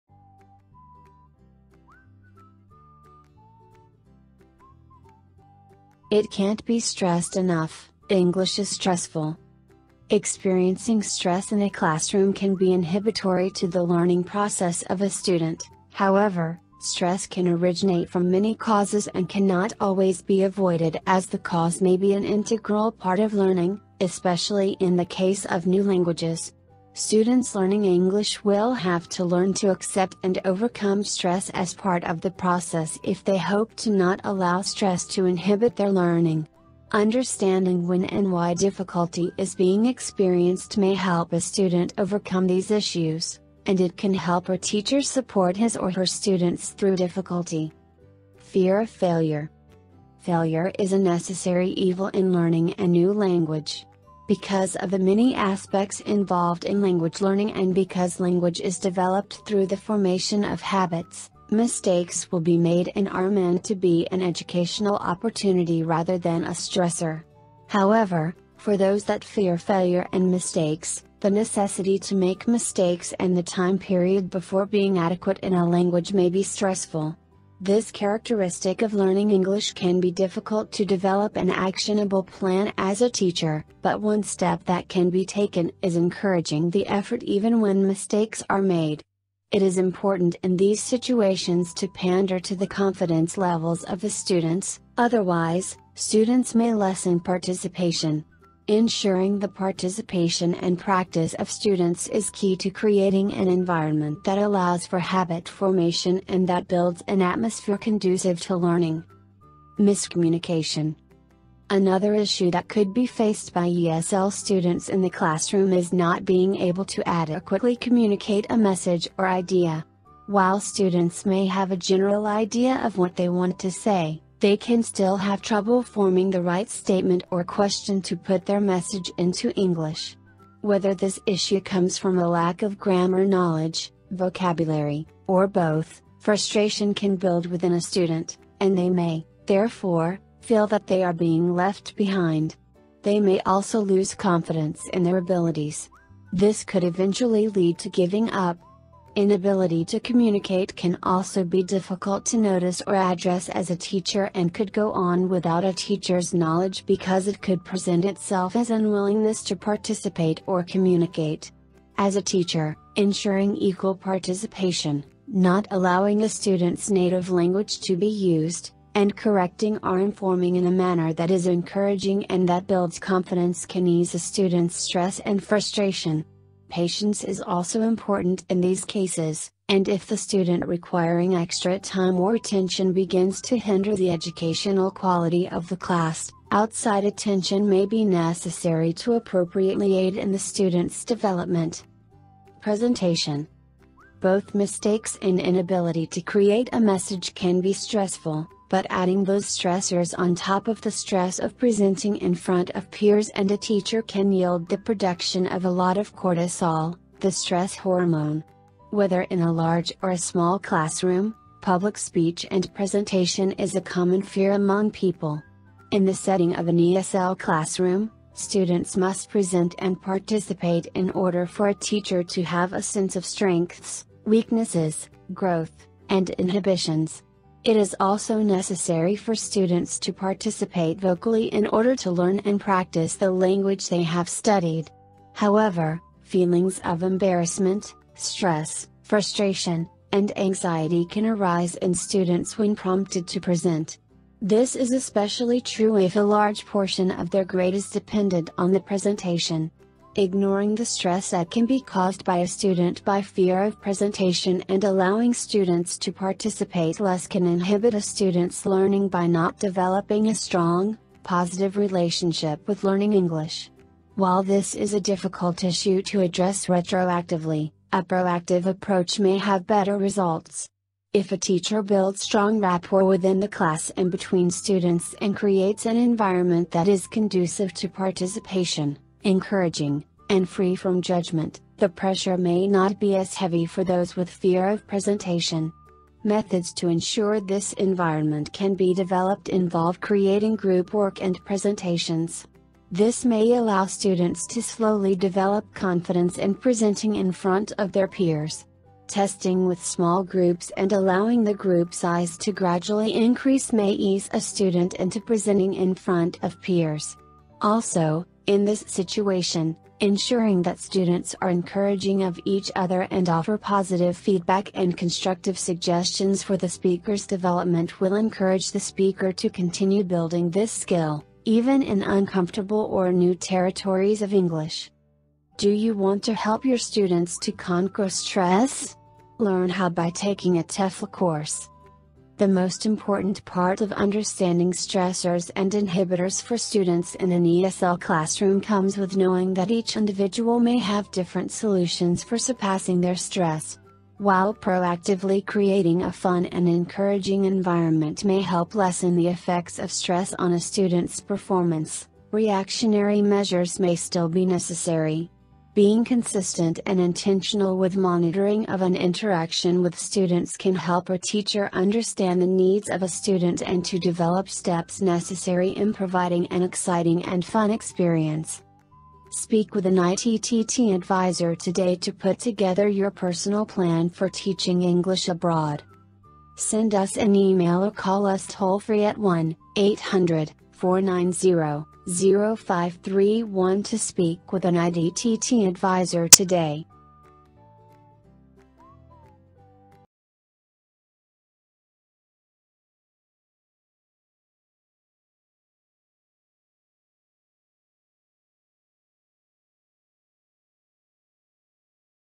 It Can’t Be Stressed Enough: English Is Stressful | ITTT TEFL BLOG
Experiencing stress in a classroom can be inhibitory to the learning process of a student; however, stress can originate from many causes and cannot always be avoided as the cause may be an integral part of learning, especially in the case of new languages. Students learning English will have to learn to accept and overcome stress as part of the process if they hope to not allow stress to inhibit their learning. Understanding when and why difficulty is being experienced may help a student overcome these issues, and it can help a teacher support his or her students through difficulty.